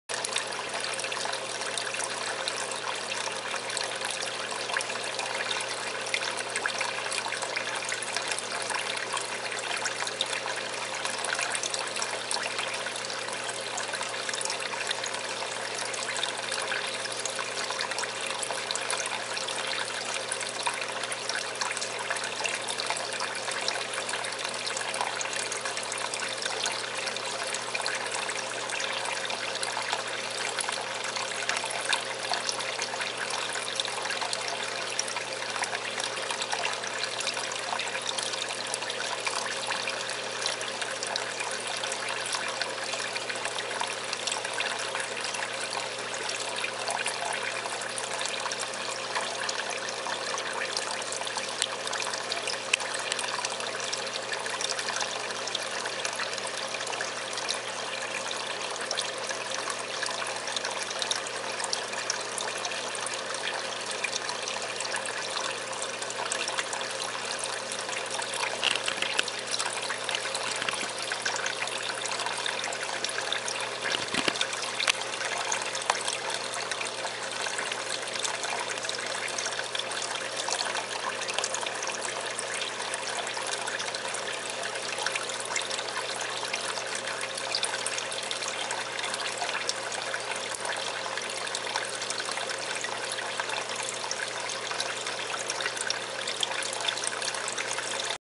Ornate / Macro Terrapins
They are waiting for their Reptomin breakfast when I turn the lights on in the morning!